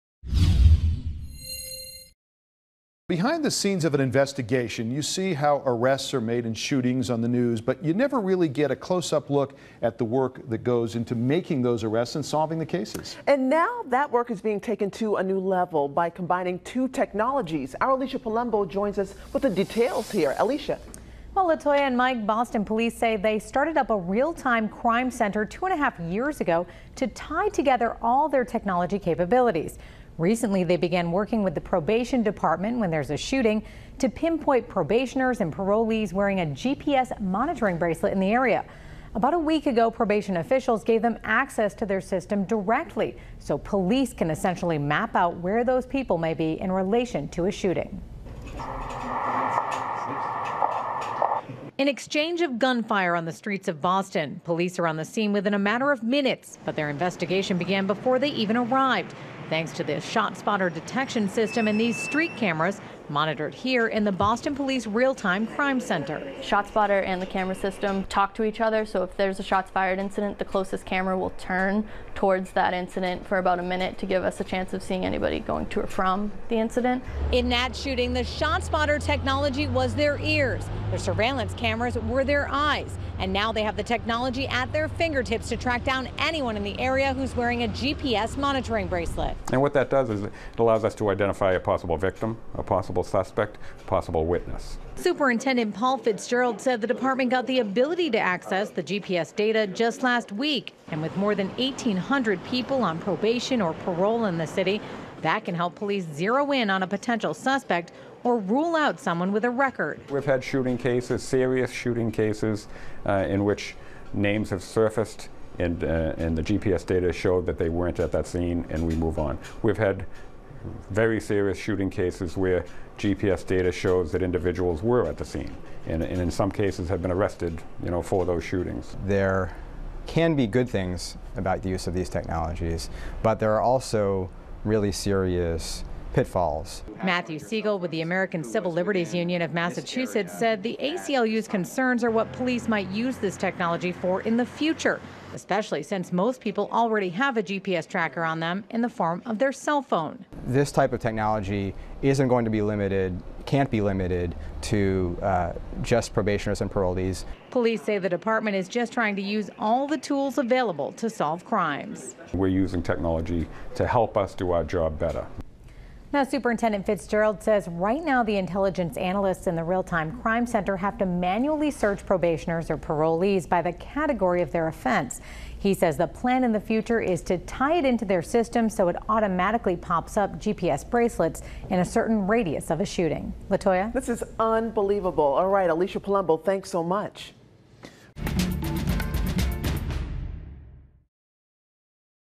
ShotSpotter, GPS, Cameras help Boston Police Combat Crime
See the innovative ways the Boston Police Department are using the ShotSpotter gunfire data in combination with GPS location data from monitoring bracelets on parolees and probationers to improve investigations.